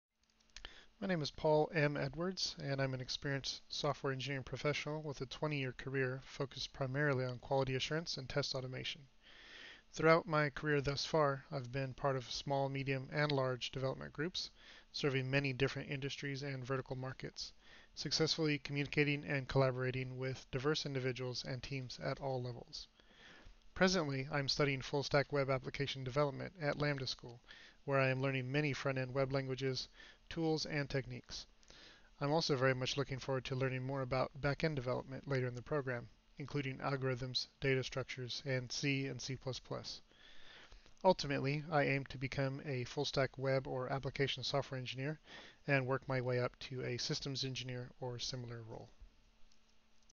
About Me
I am an experienced Software Engineering professional with a 20+year career focused primarily on Quality Assurance & Test Automation.

Throughout my career thus far, I've been part of small, medium, and large development groups, serving many different industries & vertical markets, successfully communicating & collaborating with diverse individuals & teams at all levels.

Presently, I'm studying Full Stack Web Application development at Lambda School where I am learning many front-end web languages, tools, and techniques. I'm very much looking forward to learning more about back-end development, including algorithms, data structures, and C/C++.

Ultimately, I aim to become a Full Stack Web or Application Software Engineer and work my way up to a Systems Engineer or similar role.